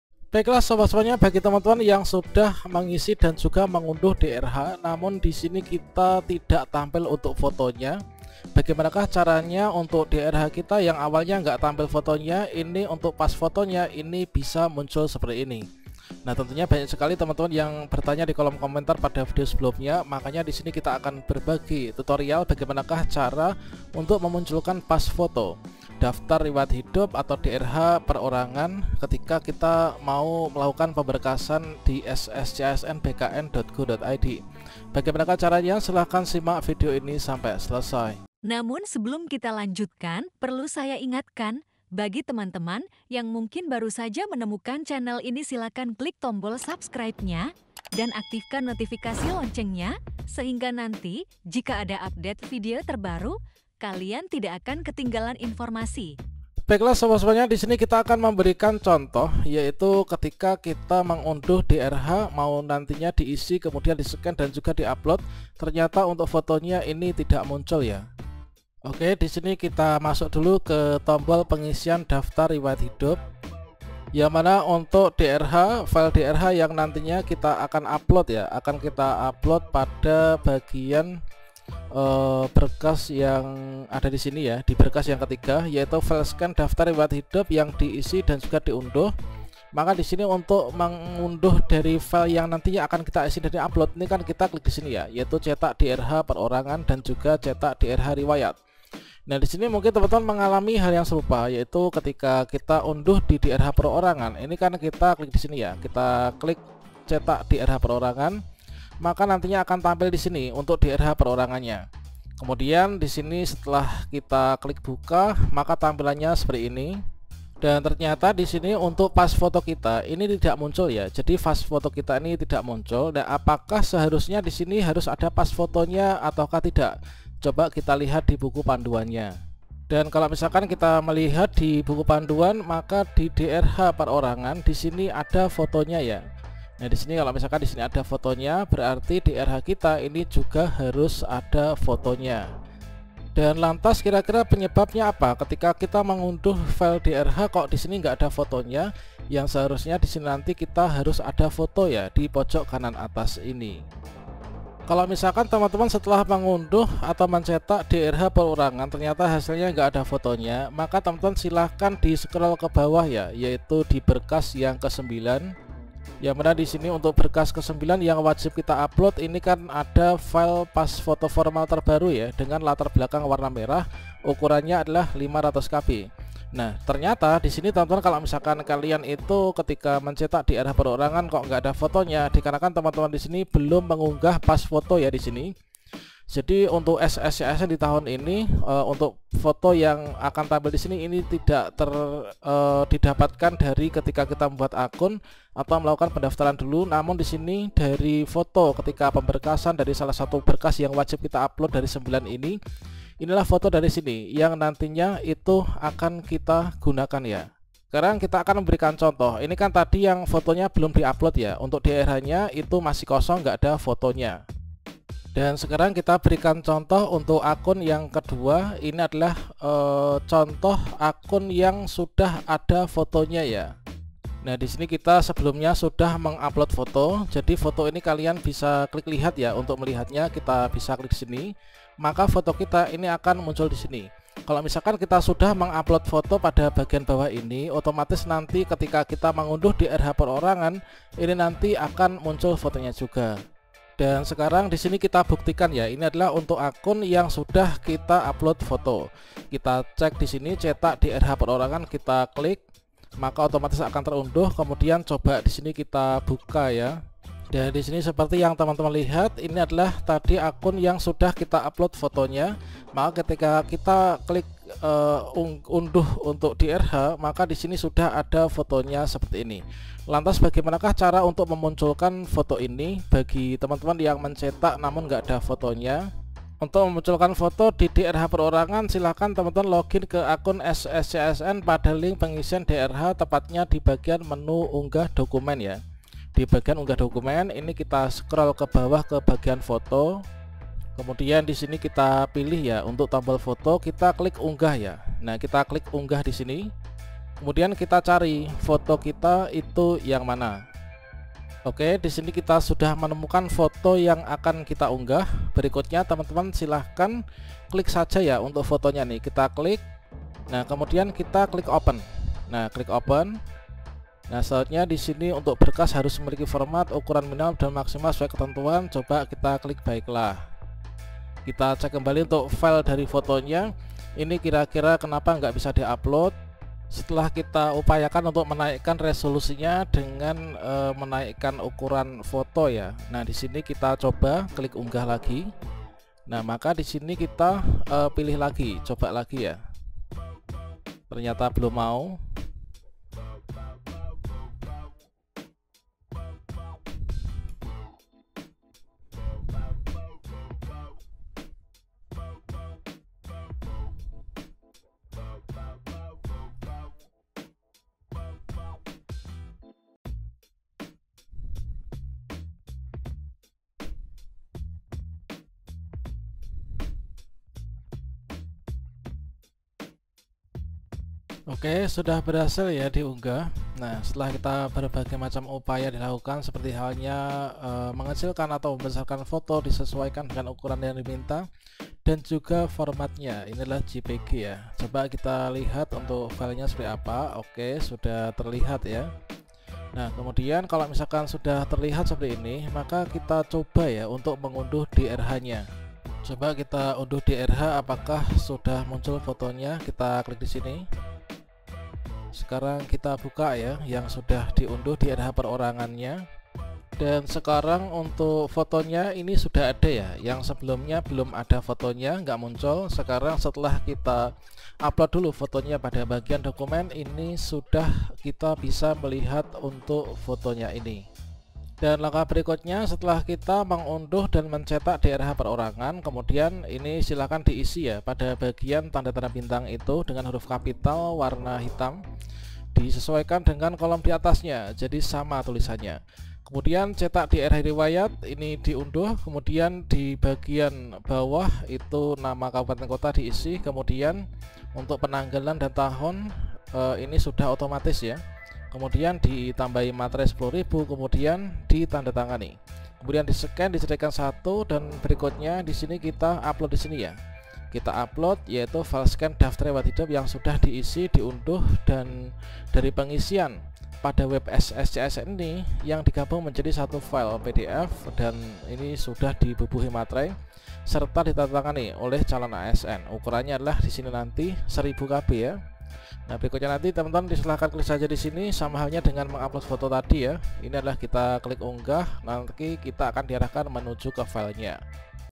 SOLUSI Pas Foto Tidak Muncul di Daftar Riwayat Hidup (DRH) Perorangan Pemberkasan PPPK Guru 2023
Cara Memunculkan Pas Foto di Daftar Riwayat Hidup DRH Perorangan PPPK Guru 2023